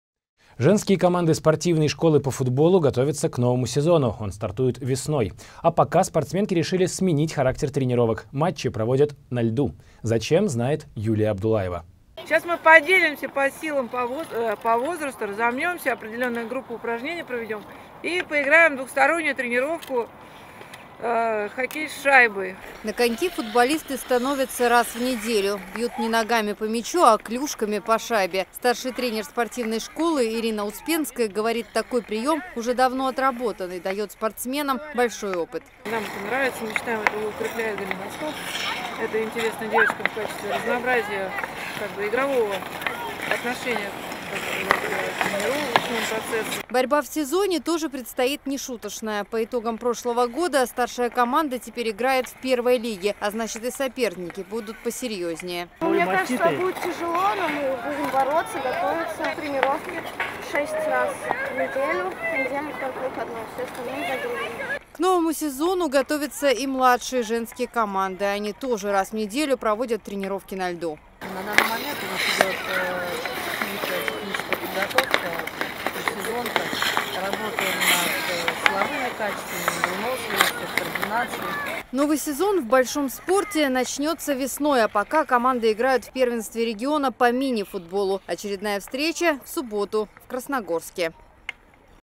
ЖЕНСКАЯ КОМАНДА ПО ФУТБОЛУ ГОТОВИТСЯ К НОВОМУ СЕЗОНУ 11.02.15
Женские команды спортивной школы по футболу готовятся к новому сезону - он стартует весной. А пока спортсменки решили сменить характер тренировок. Матчи проводят на льду. Зачем, узнала наша съемочная группа.